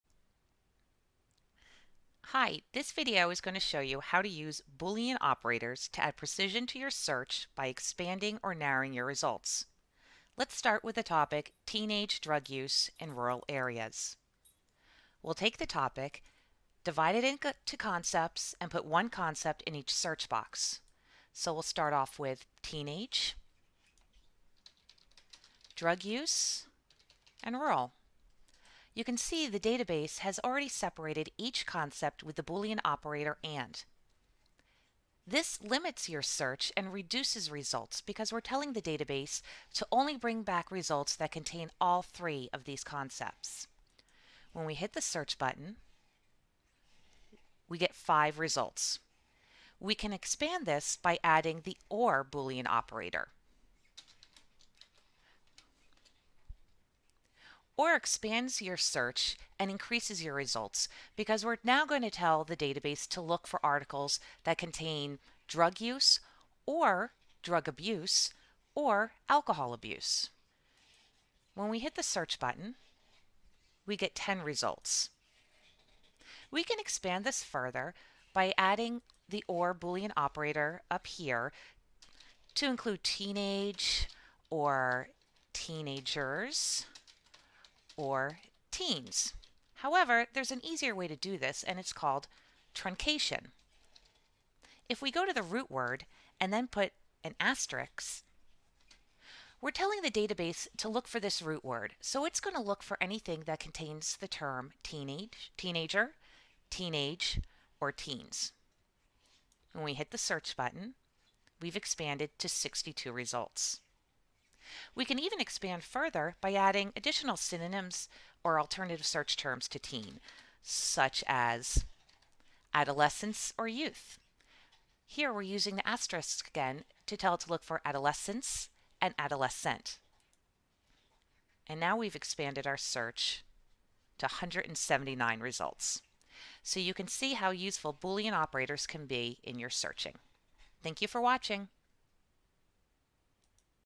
Boolean Operators and Truncation
Sample search in database using Boolean operators OR and AND. Demonstrates truncation of a root word to expand search results.